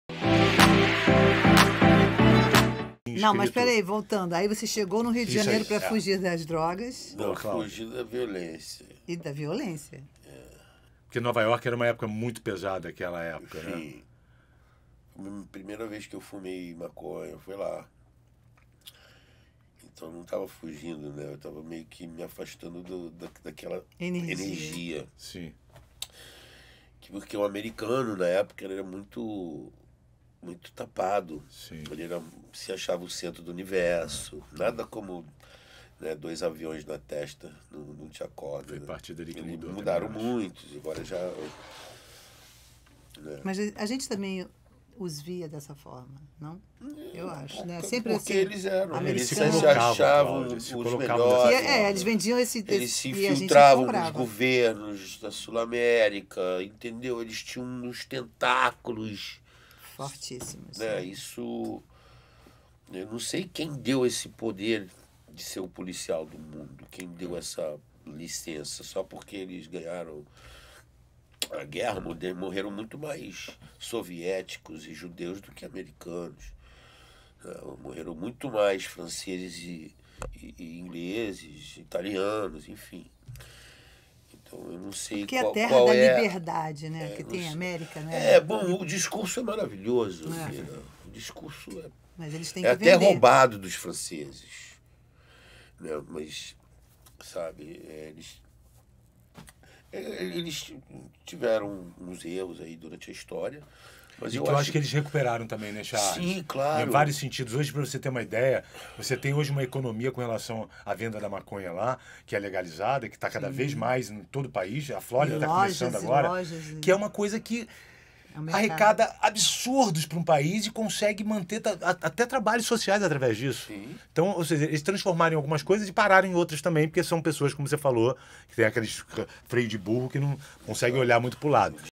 Charles Paraventi cita a soberba dos Estados Unidos em relação aos outros países.
Charles Paraventi fala dos tentáculos que a terra do Tio Sam tem em relação ao resto do mundo, de  como foi seu primeiro contato com as drogas e seu afastamento da violência.

ANFITRIÕES (1º temporada):
-Anderson Müller
-Cláudia Lira